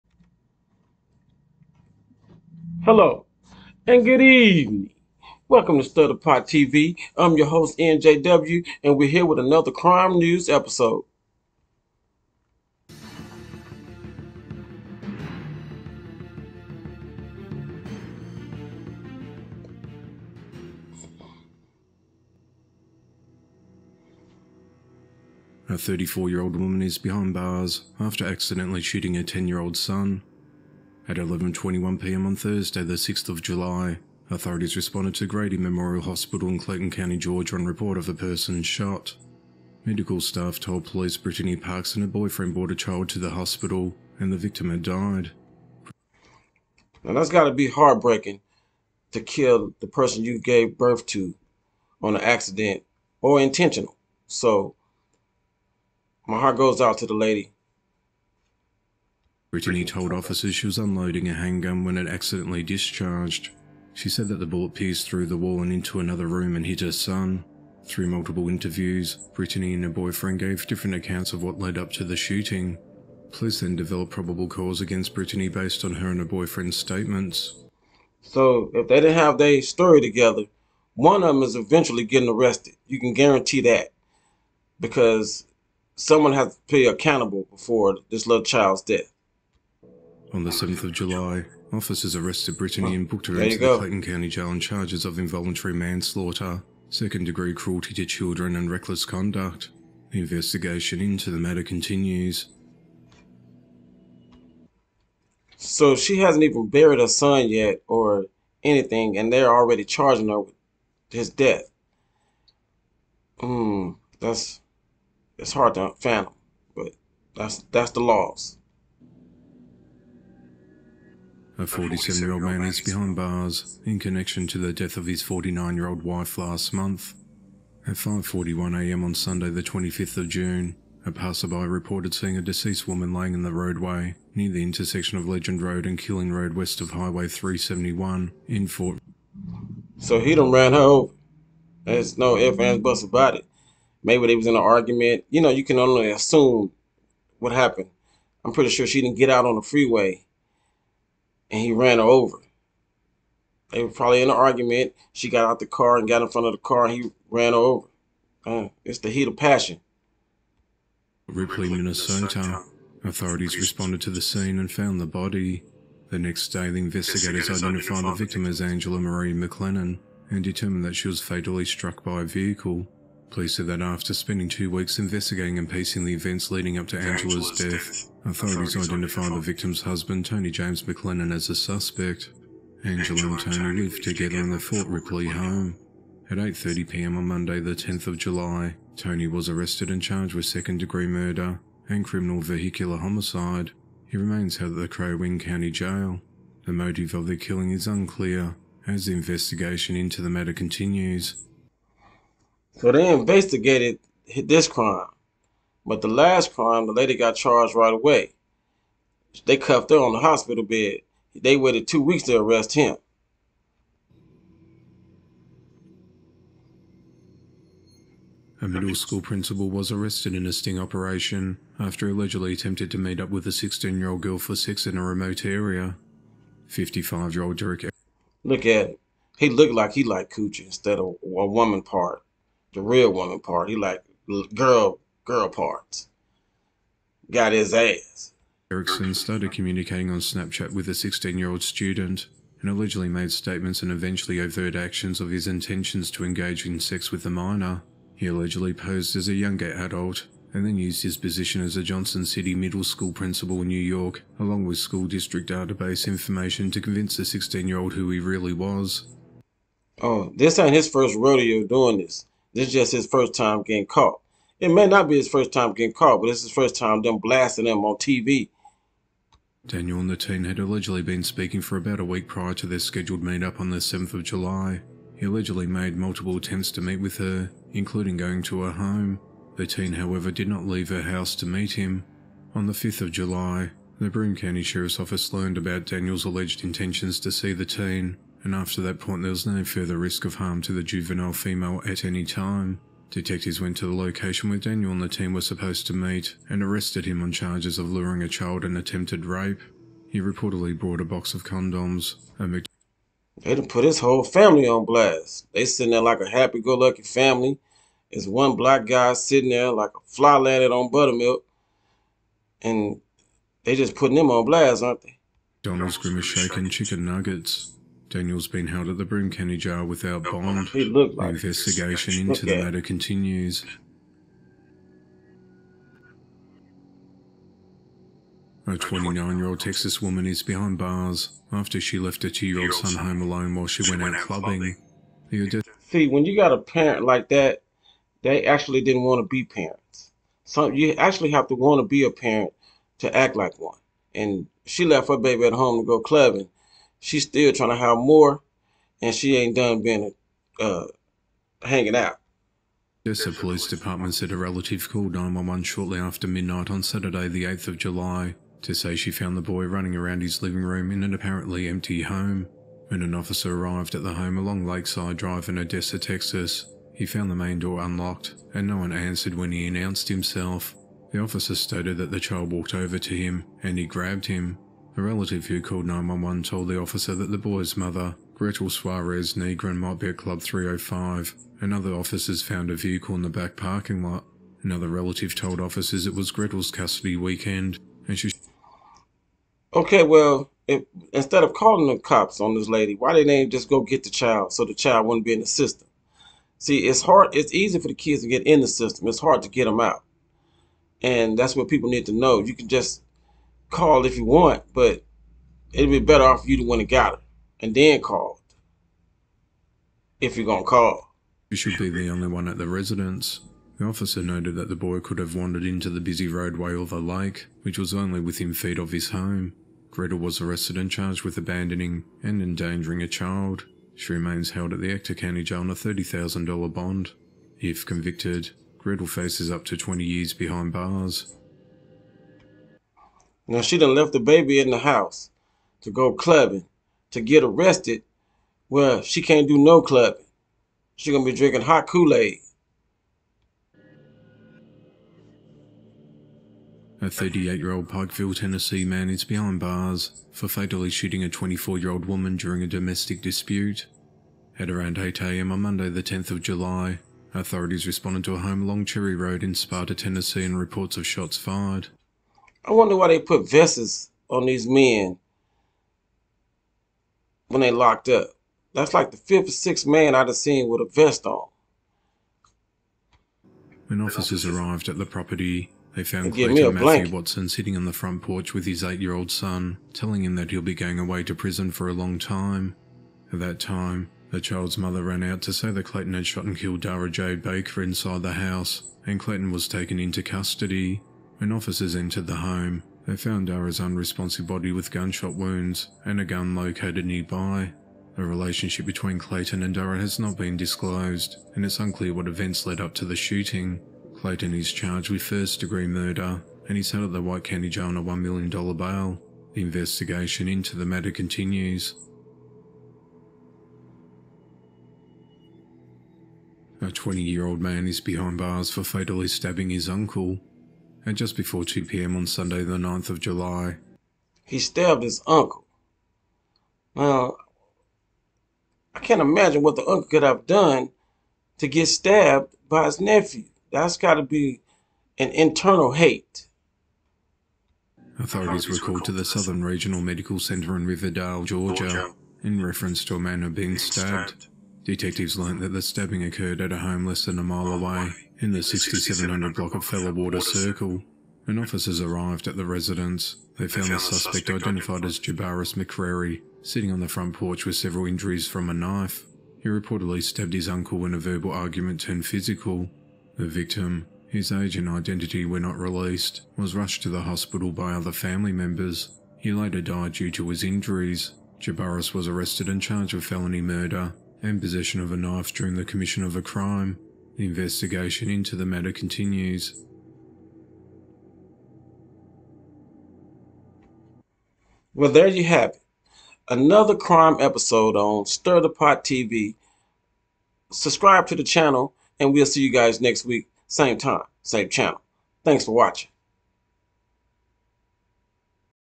The Hidden Dangers of Excessive Love & Guilt
Do you ever feel overwhelmed by love and guilt? In this video, we'll explore the hidden dangers of these powerful emotions and how they can affect our relationships and well-being.

From crime news to personal anecdotes, we'll dive into the effects of excessive love and guilt on individuals and society. This is a must-watch for anyone looking to understand the impact of these emotions and how to manage them in a healthy way.